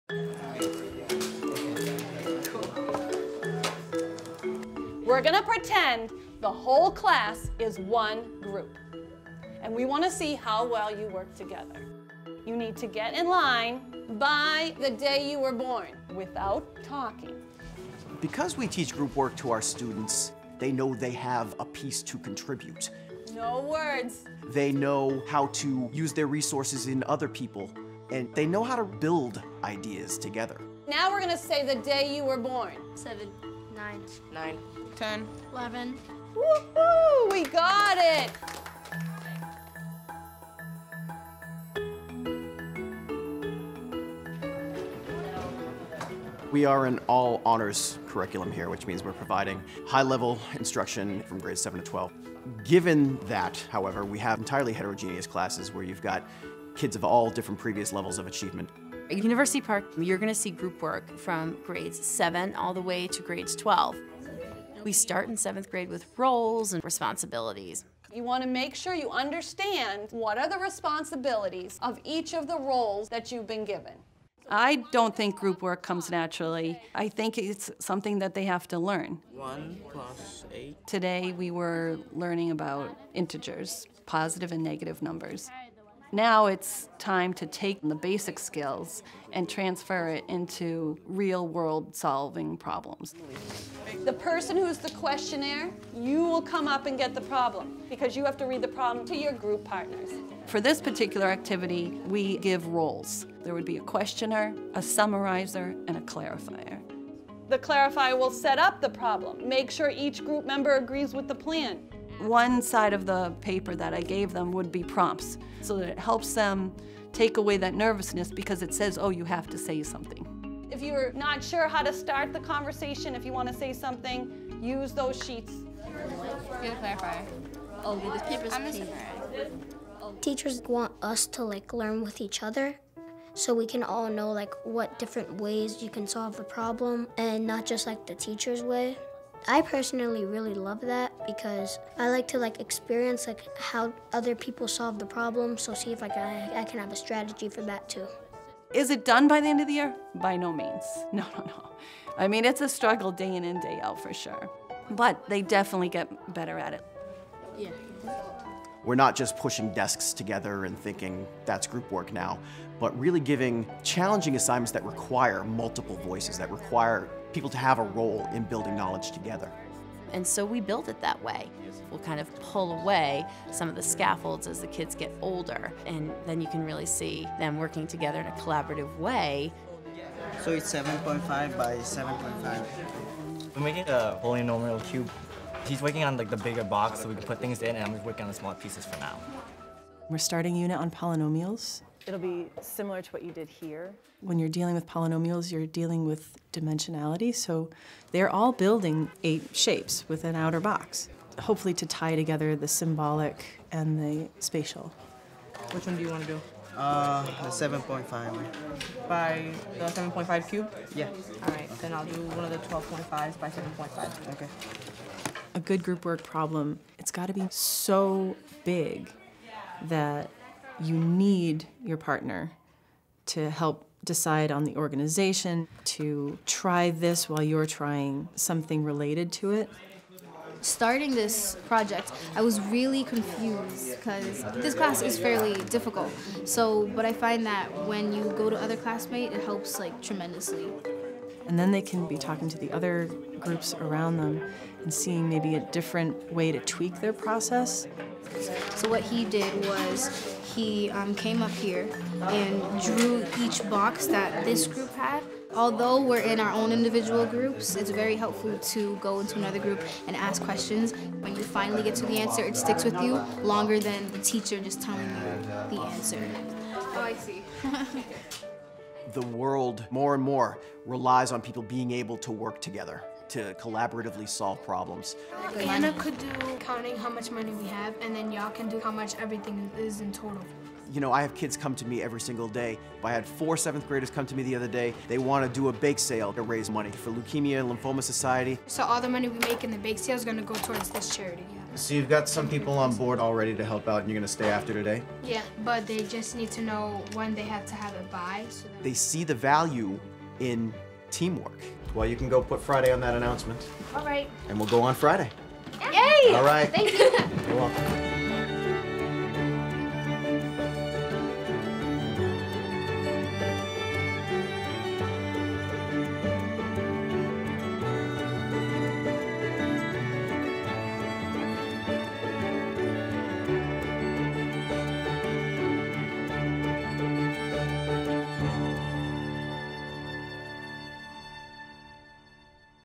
Teaching Group Work: Building Student Collaboration and Agency
At University Park Campus School, students learn through group work that they have something to contribute.

University Park Campus School
GRADES 7-12 | WORCESTER, MA